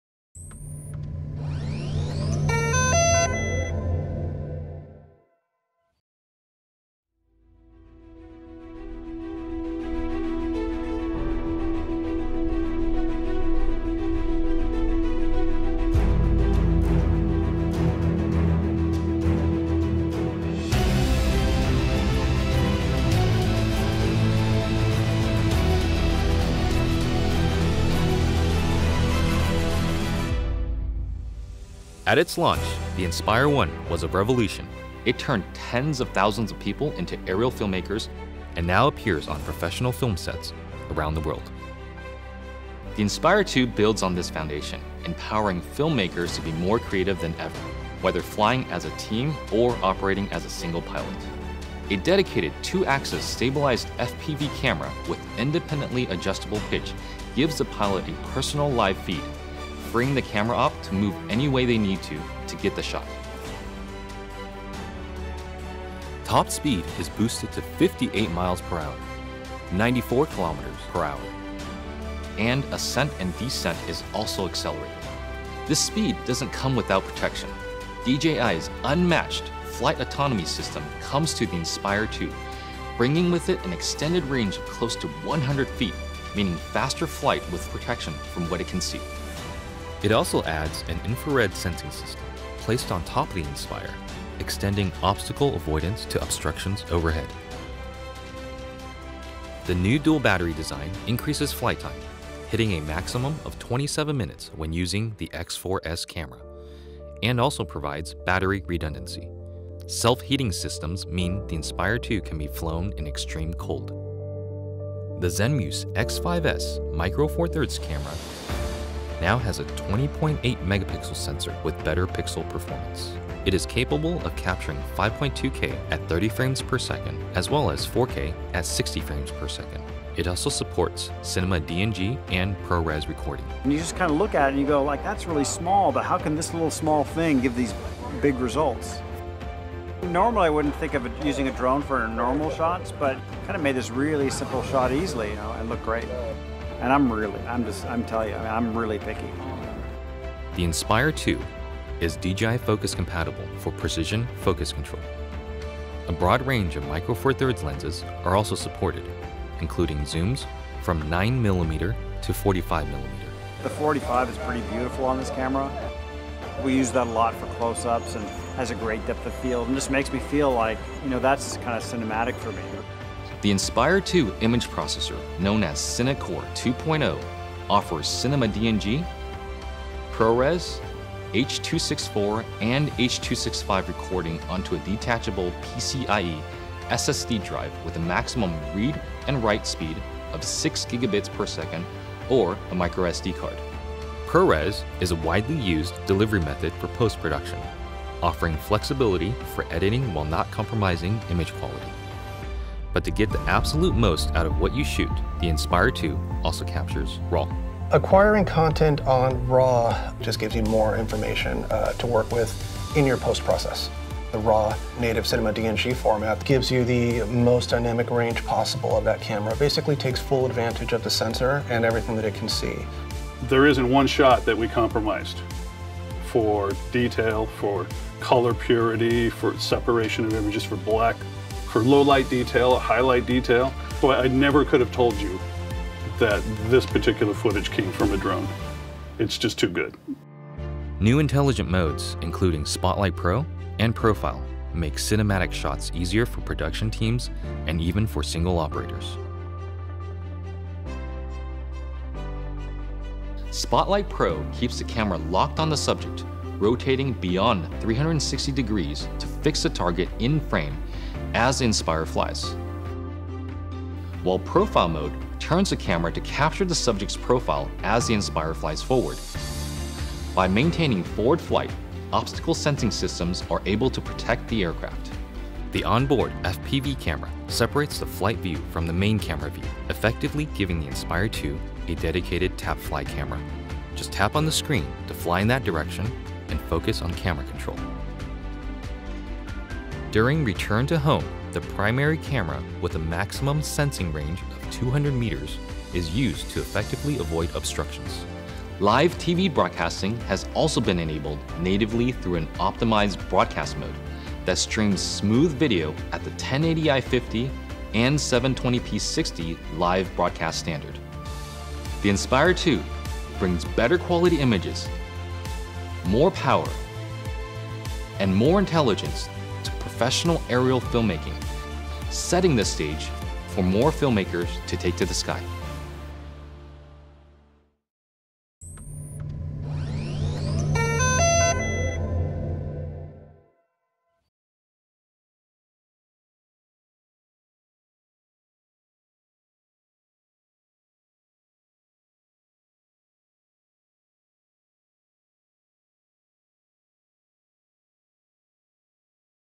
Introducing the Inspire 2 Explained
The DJI Inspire 2 is the next evolution of professional aerial cinematography. New cameras, sensors, and intelligent features make the Inspire 2 the definitive aerial cinematography solution.

Always follow applicable rules and regulations in your jurisdiction. Permits were obtained, where needed, in locations depicted in this video.

Dear DJI Customers:

We recently announced the Inspire 2, a ready-to-fly aerial platform for high-end filmmaking and video creators. At launch, we said the Inspire 2 is expected to accelerate to 80 kmph in 4 seconds and reach a top speed of 108 kmph.

After further optimization and testing of various components of the craft, camera gimbal and propulsion system, our engineers have revised those specifications. The Inspire 2 is now expected to accelerate to 80 kmph in 5 seconds and reach a top speed of 94 kmph. These changes are necessary to ensure speed does not compromise video quality and stability.

We believe the Inspire 2 is the world’s most powerful and versatile aerial platform for professional filmmakers and content creators. However, we realize this small change in specifications may be significant to those who want to push their visions to the limit. Any customers who have pre-ordered an Inspire 2 but no longer wish to purchase one will be able to request a full refund from DJI and its authorized resellers. We will email all Inspire 2 customers to make them aware of this change and the refund process.

Sincerely,
DJI